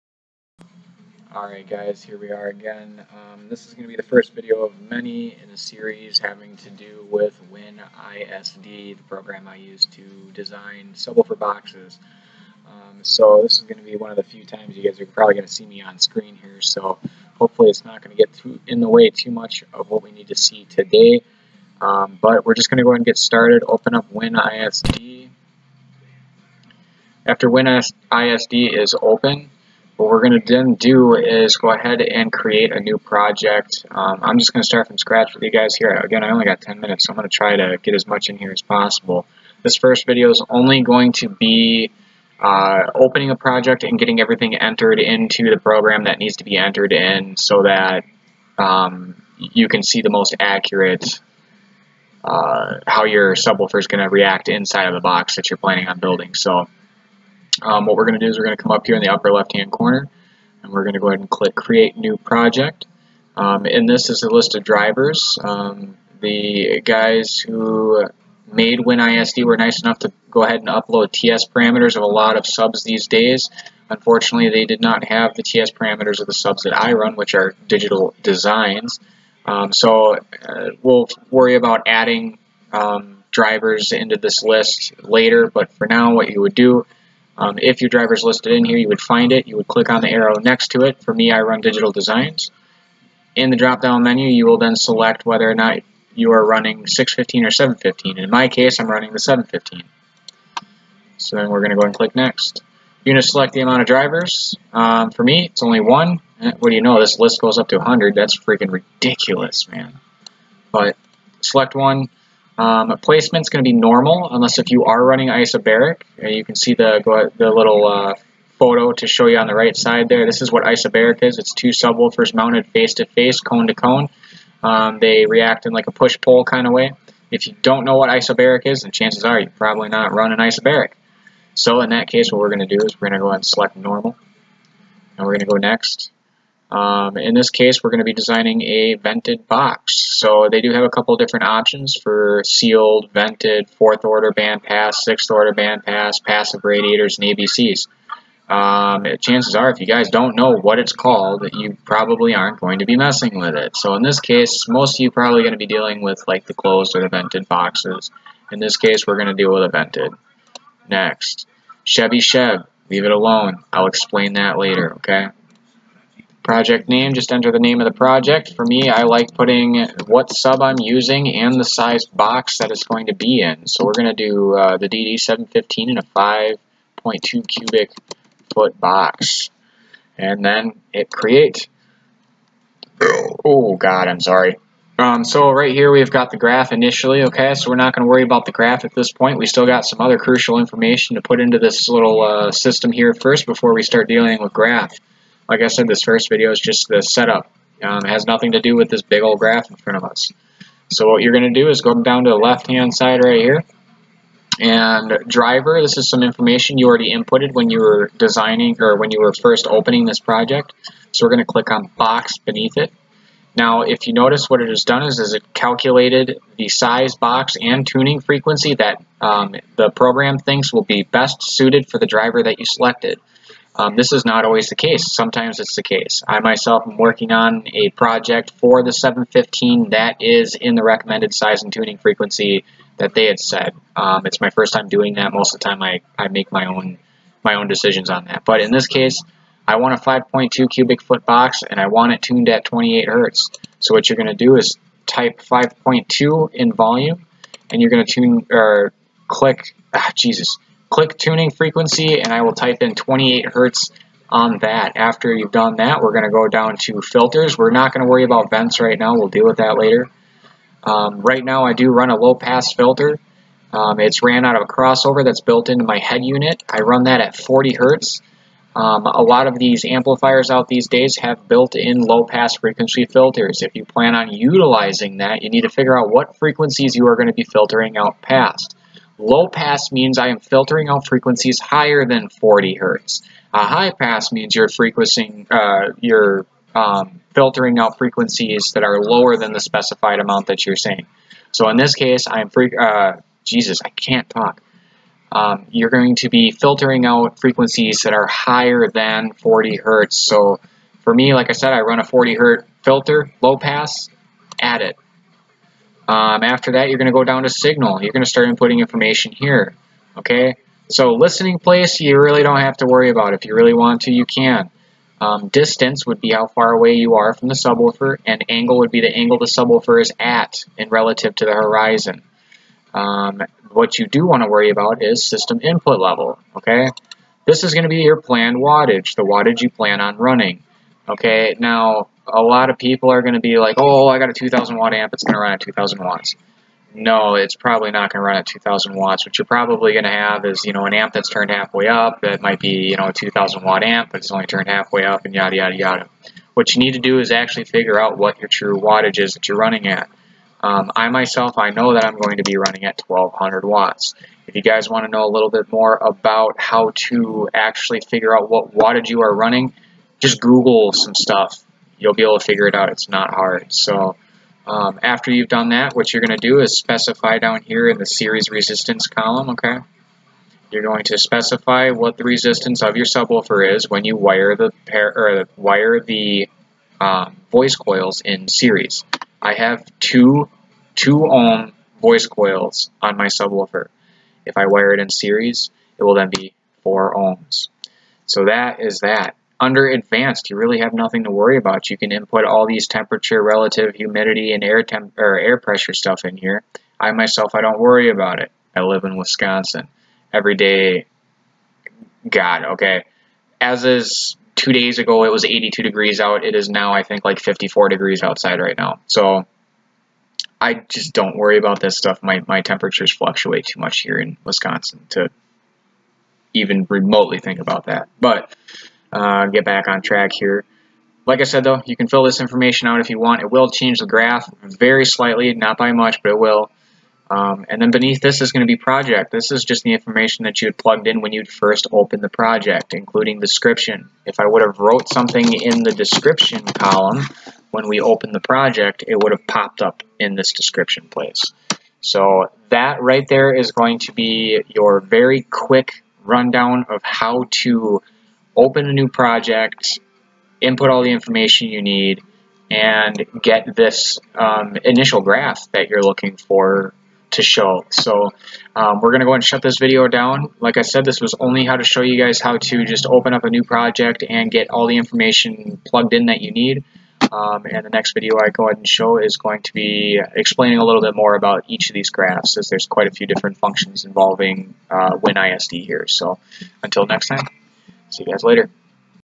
Winisd tutorial Episode 1 How to open new project
In this video i describe how to open a new project in winisd and enter some important information that is needed. This is one of many videos that are to come in this Series of videos.
Winisd is NOT a program i designed, i am only one of many people who utilize this program.

I apologize for the kinda crappy audio quality, i hope soon to make it a bit better.

ATTENTION
I stand corrected, the "Series Resistance" Under Signal Source should remain at .1 ohm